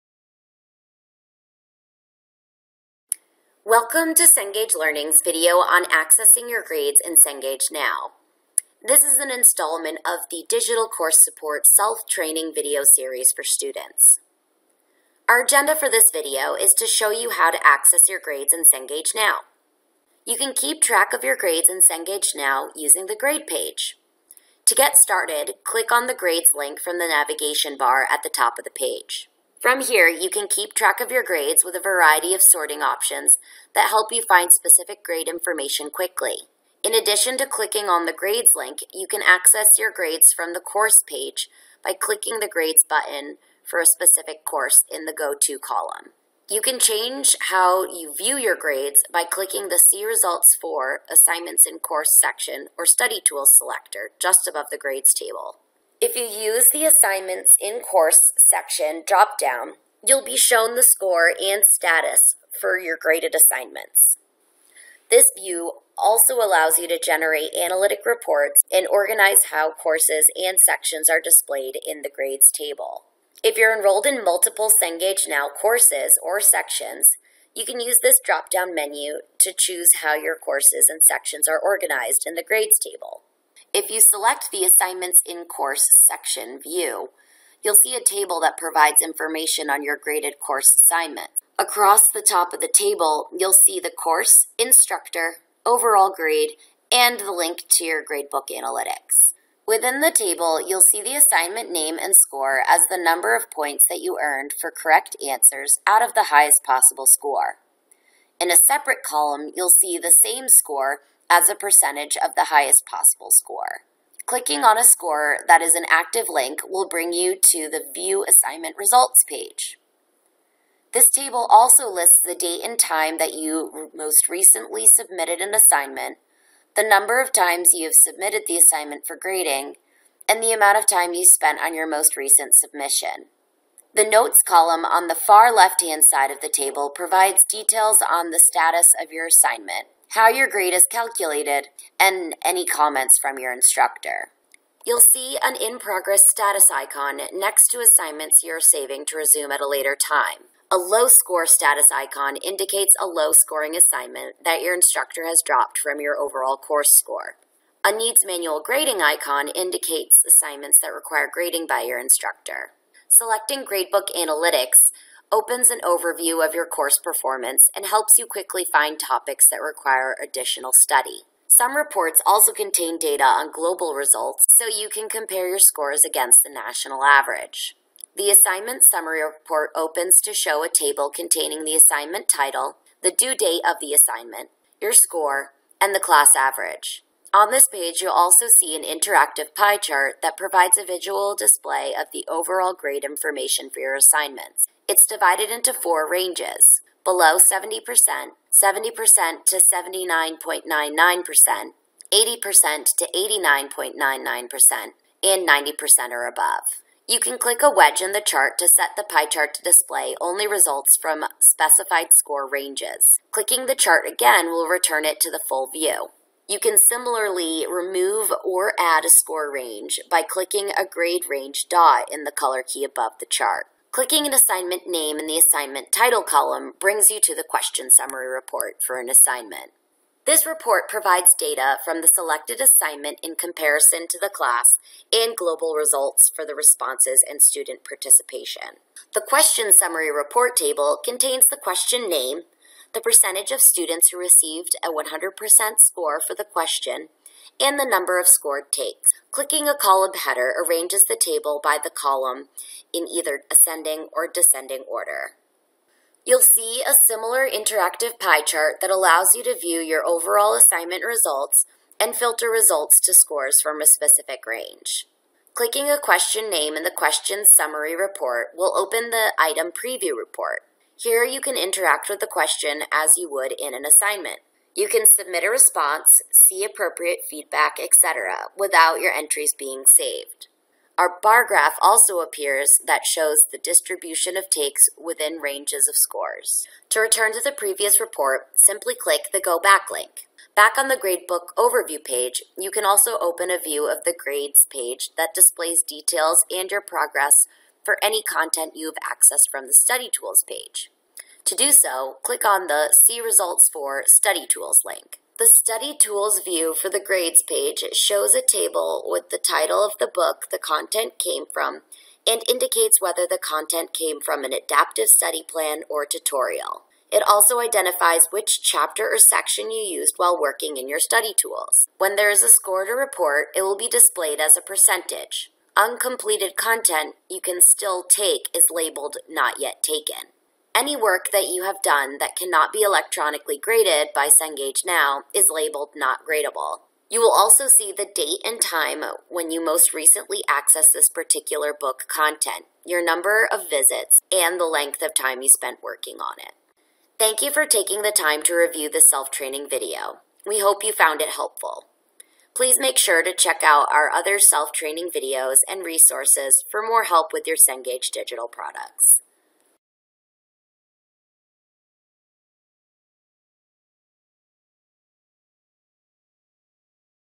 CengageNOWv2: Accessing Your Grades
This student resource video covers accessing your grades in CengageNOWv2.